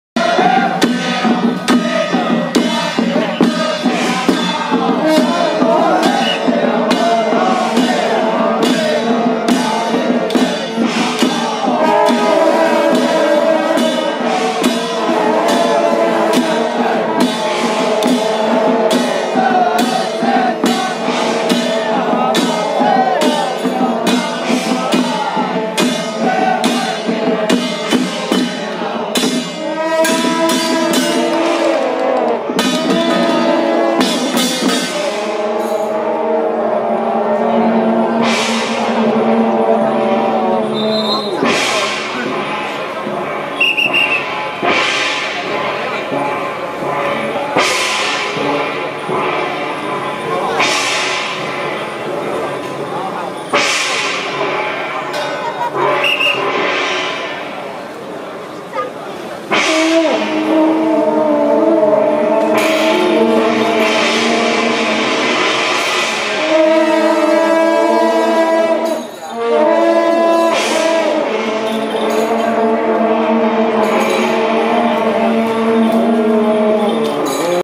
104乙未年艋舺河溝頭晉德宮繞境【北邑永聖社哨角團】
2015/11/13艋舺河溝頭晉德宮助順將軍聖誕遶境